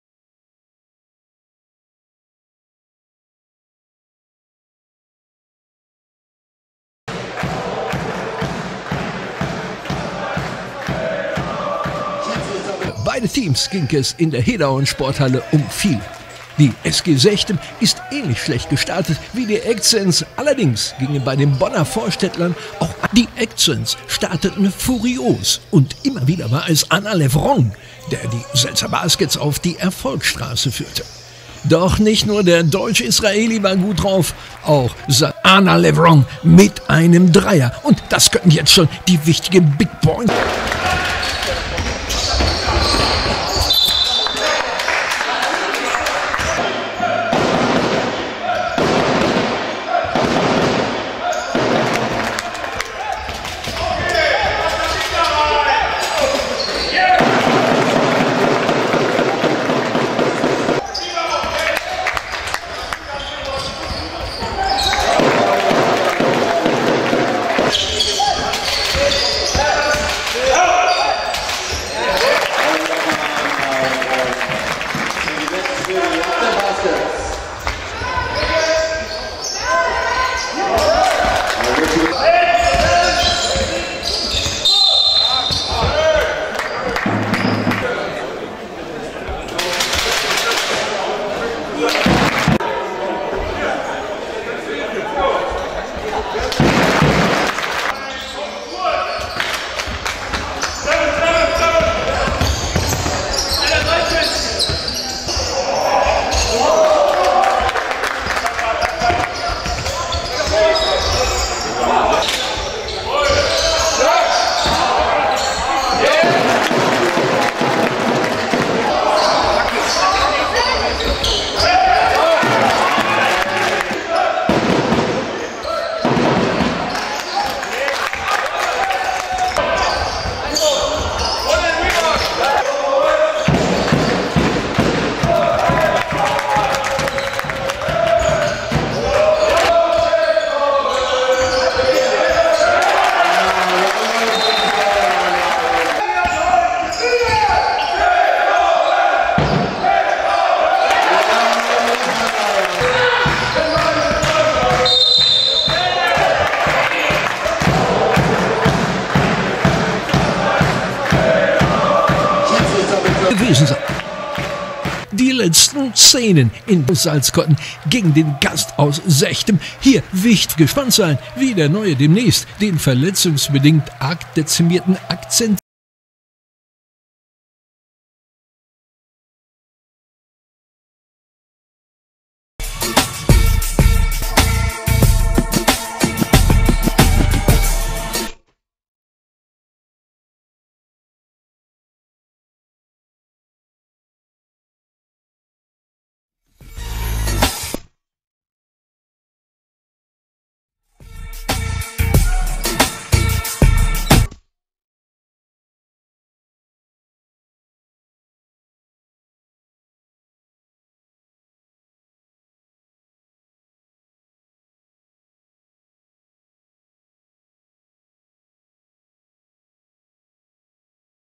Aner Levron #14
TV 1864 Salzkotten vs SG Sechtem  86:82   + Bonus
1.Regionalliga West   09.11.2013
#14 Aner Levron 24 points 10 Reb